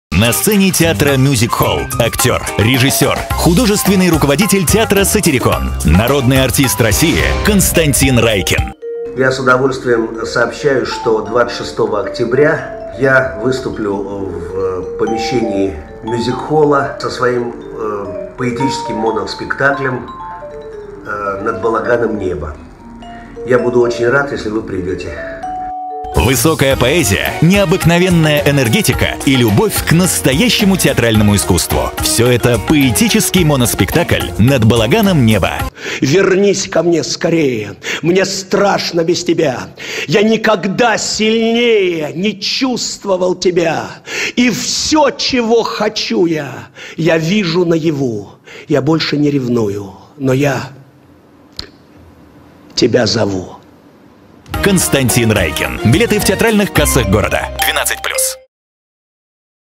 НА БИС! 26 октября - К.А. Райкин - моноспектакль "Над балаганом небо..." kassir.ru
ГЕНИАЛЬНЫЙ АРТИСТ, ВЕЛИКИЙ КОНСТАНИН АРКАДЬЕВИЧ РАЙКИН В ПЕТЕРБУРГЕ - 26 ОКТЯБРЯ 2018 - Мюзик Холл

Константин Аркадьевич Райкин читает стихи любимых поэтов. Со сцены звучат стихи Давида Самойлова, Николая Заболоцкого, Николая Рубцова, Осипа Мандельштама, Лопе де Вега, Александра Сергеевича Пушкина.

Константин Аркадьевич РАЙКИН - художественный руководитель Государственного театра «Сатирикон» им Аркадия Райкина, народный артист России, режиссер, лауреат Государственных премий РФ, четырежды лауреат Национальной премии «Золотая Маска», лауреат премии Фонда К.Станиславского, премий «Хрустальная Турандот», «Чайка», «Кумир», «Триумф», художественный руководитель «Высшей школы сценических искусств», профессор.
Поэтический моноспектакль «Над балаганом небо…» — это высокая поэзия, пропущенная через сердце.